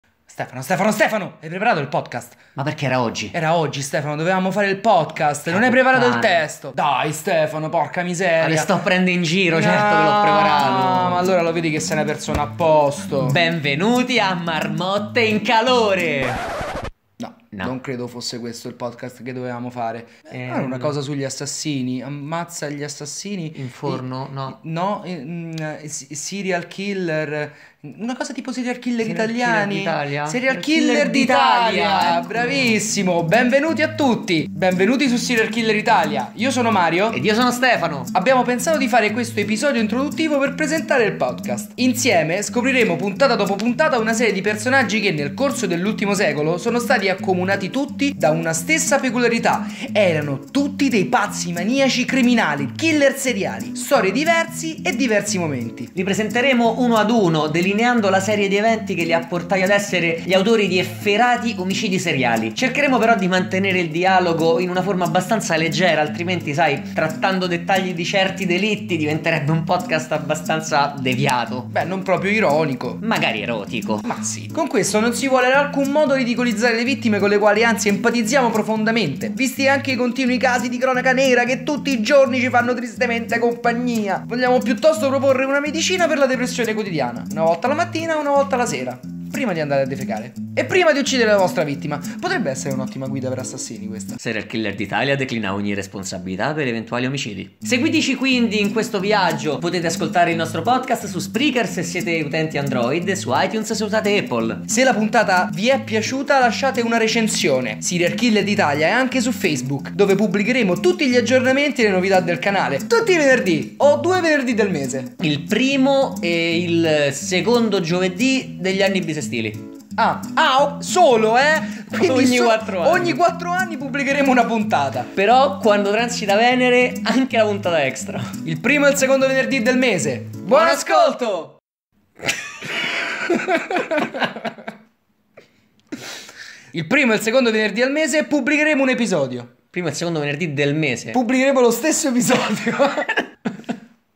00. Episodio Introduttivo - Serial Killer d'Italia
In questo episodio presentiamo il podcast di Umorismo Criminale, che all’inizio si chiamava Serial Killer d’Italia.

E’ un episodio dalla qualità audio pessima (non avevamo ancora comprato il microfono), con un montaggio talmente terribile che sembra fatto con l’accetta da un boscaiolo drogato di crack.

Se riuscirai a sopportare quest’ammasso di schifo (che, mi dispiace per voi, proseguirà ancora nei primi episodi), entrerai in un mondo di perdizione, lasciandoti mangiare l’anima, a poco a poco, dal nostro delirante black humor.
Buon ascolto!

Il primo podcast Black Humor di cronaca nera che consumerà il tuo ultimo brandello di morale!